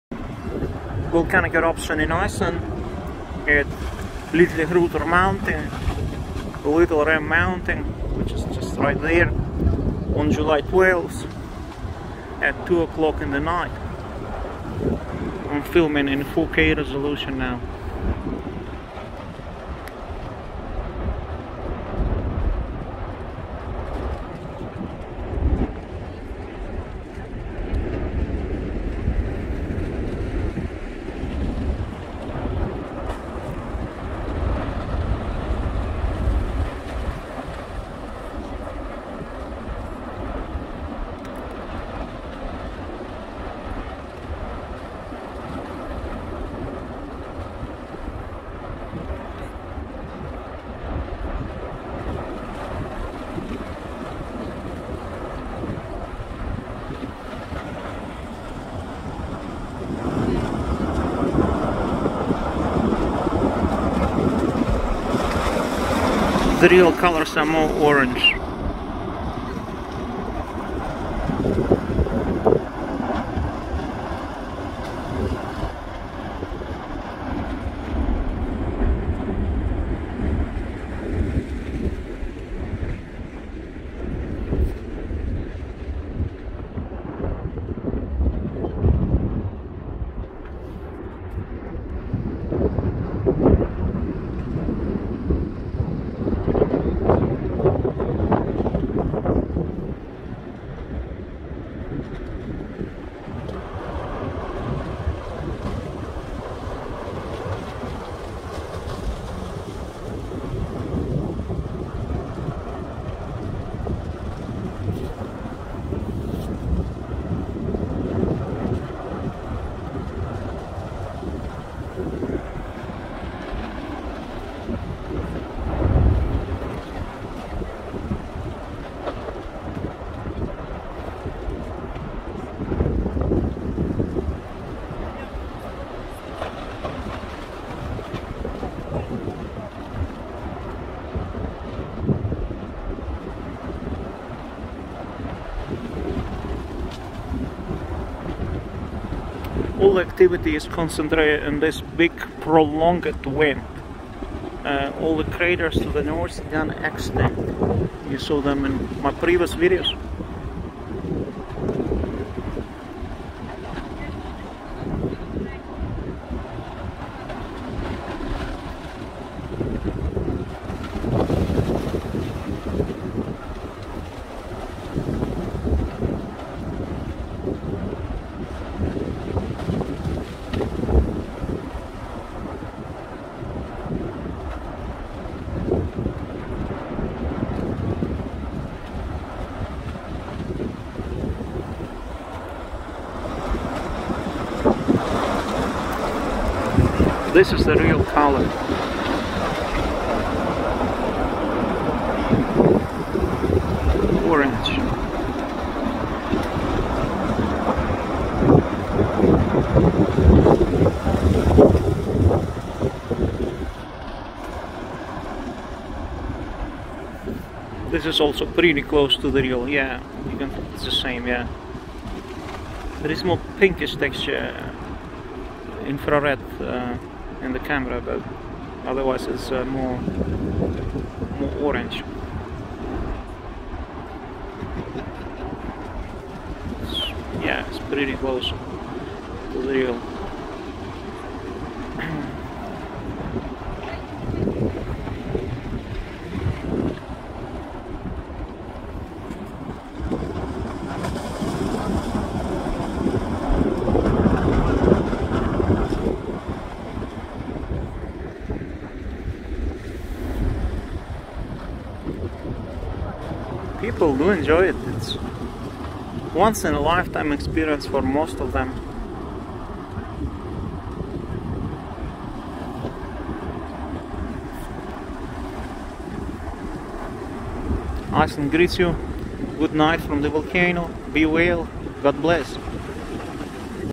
Massive Cone of New Iceland Volcano builds up super fast 🌋Standing next to it! Watch in 4K! 12.07.23
Observe erupting cone from meters away. We are standing right next to the erupting volcano in Iceland. Filmed in 4K resolution at 2 am at night on July 12th, 2023.